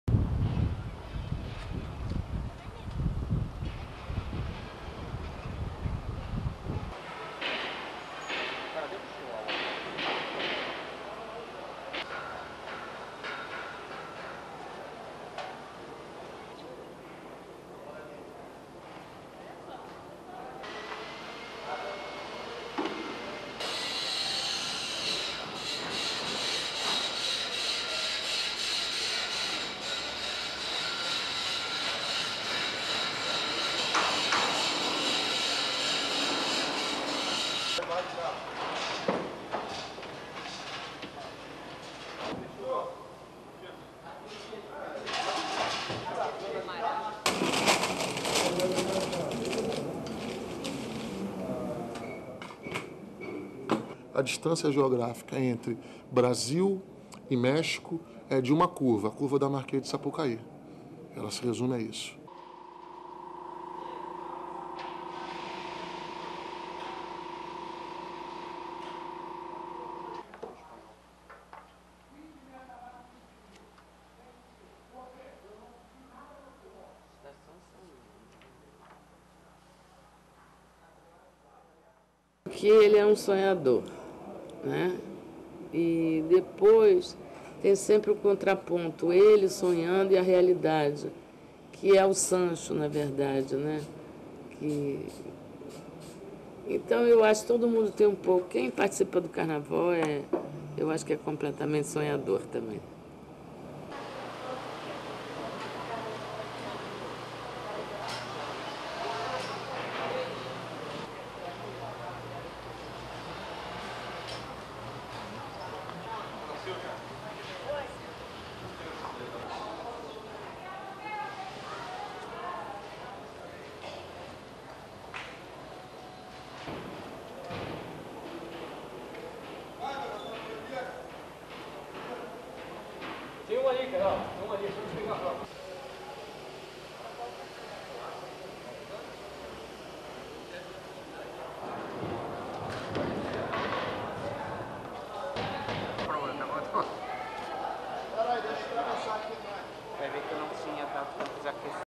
Samba schools find inspiration in art and literature for this year''s carnival
1. Pan of Samba City venue
2. Wide of Viradouro Samba School floats in workshop
3. Close of giant chihuahua head on float, man working underneath
4. Wide of Day of the Dead float
5. Model of skull
6. Various of models of artists Frida Kahlo and Diego Rivera
7. Men working on Mexican-themed float with statue of Revolutionary Leader Emiliano Zapata
8. Close up of models of chilies on float
9. Viradouro Samba School Theme Designer Junior Schall working on design
10. SOUNDBITE: (Portuguese) Junior Schall, Viradouro Samba School Theme Designer:
"The geographical distance between Mexico and Brazil has been reduced down to a curve, the curve at the entrance of Sapucai Avenue (the Sambadrome)."
11. Close of sign reading (Portuguese) "Don Quixote of la Mancha, the knight of impossible dreams" with a Brazilian and Spanish flag, zoom out to people working on floats in Uniao da Ilha Samba School workshop
12. Uniao da Ilha Theme Designer Rosa Magalhaes looking through book
13. Close of Magalhaes turning pages of illustrated version of Don Quixote de La Mancha book
14. SOUNDBITE: (Portuguese) Rosa Magalhaes, Uniao da Ilha Samba School Theme Designer:
"He (Don Quixote) is a dreamer who constantly has to face reality, which is represented by Sancho Panza. Those of us who work in carnival, we are all dreamers."
15. Wide of workshop with Spanish-style costume on mannequin in foreground
16. Close of mannequin
17. Various of people sewing costumes
18. Various of models of windmills that will be part of float
19. Tilt down of carnival float with models of knights
20. Medium of man painting glue on top of brocade on float
21. Close of models of knights
22. Wide of people working on float
STORYLINE:
Rio de Janeiro''s top samba schools have been gathering inspiration from across the globe for their carnival parades this year.
Tourists and spectators visiting the famous carnival in Brazil will be treated to a spectacle celebrating art and literature from different parts of the world.
The Viradouro Samba School has taken inspiration from Mexico, spicing up their 82 minute show with floats reminiscent of the country''s traditional Day of the Dead celebration, as well as hanging chilli peppers, tequila bottles and sombrero hats.
Iconic Mexican figures such as revolutionary leader Emiliano Zapata, and artists Frida Kahlo and Diego Rivera will also feature in the show.
"The geographical distance between Mexico and Brazil has been reduced down to a curve, the curve at the entrance of Sapucai Avenue (the Sambadrome)," the school''s theme designer Junior Schall said.
The Uniao da Ilha school travelled a little further to find their theme: the Spanish Golden Age of author Miguel de Cervantes and the fictional land of his novel "Don Quixote."
Theme designer Rosa Magalhaes said she was inspired by the character of Don Quixote, an idealistic country gentleman who decides to dress as a knight and set out on his own chivalric quest.
He is accompanied on his adventures by his faithful sidekick, the realist Sancho Panza.
"He (Don Quixote) "is a dreamer who constantly has to face reality, which is represented by Sancho Panza. Those of us who work in carnival, we are all dreamers," she said.
Magalhaes said the parade will include several famous scenes from the book, including Don Quixote''s battle against windmills he believes to be giants.
Twelve schools are scheduled to compete in this year''s competition in the purpose-built samba "stadium" the Sambadrome on February 14 and 15.
The Uniao da Ilha school will be the first to parade; Viradouro will be the fourth.